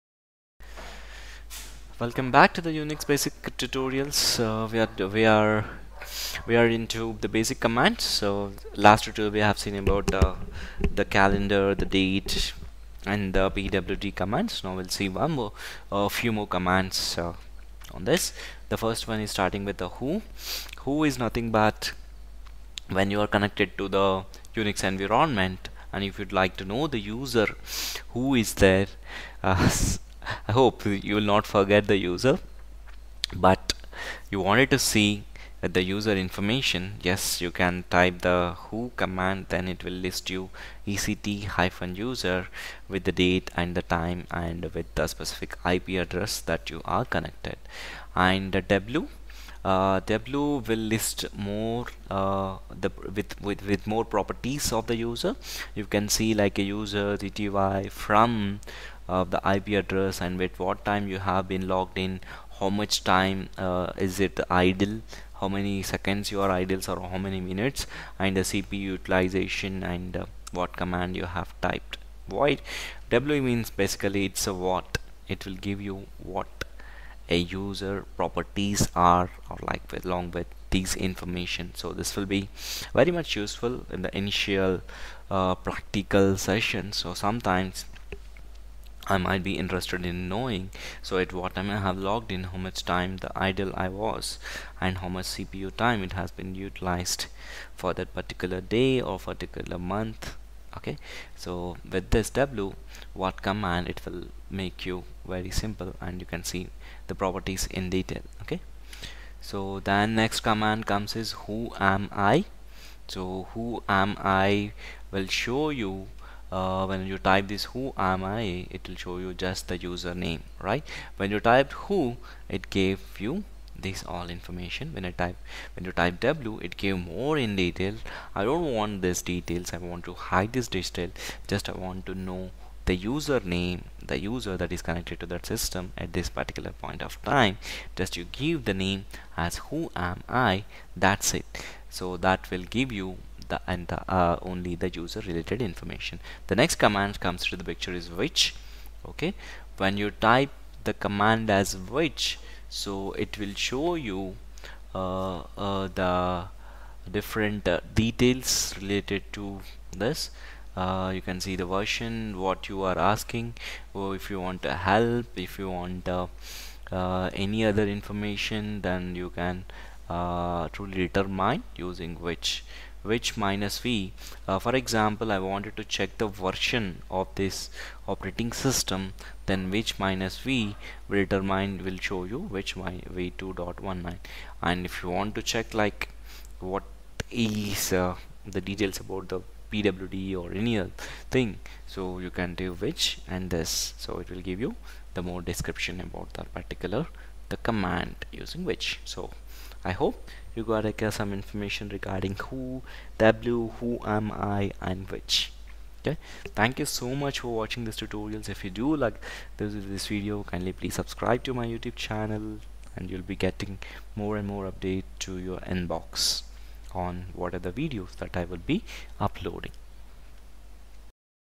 Who,W,Whoami,Which Commands in Unix - Unix Basics Tutorial - Part 14
Beginner Guide to Unix ,Unix and Unix-like operating systems are a family of computer operating systems that are derived from the original Unix System from Bell Labs.unix is a multi-user system where the same resources can be shared by different users.
unix provides multi-tasking, wherein each user can execute many processes at the same time.unix provides a hierarchical file structure which allows easier access and maintenance of data.
Unix has built-in networking functions so that different users can easily exchange information.Unix functionality can be extended through user programs built on a standard programming interface.
Here in this unix basics tutorial video you will learn about different commands in unix,Who command in unix,W command in unix,Whoami command in unix,Which Command in Unix,how to use unix commands and use of different unix commands,etc.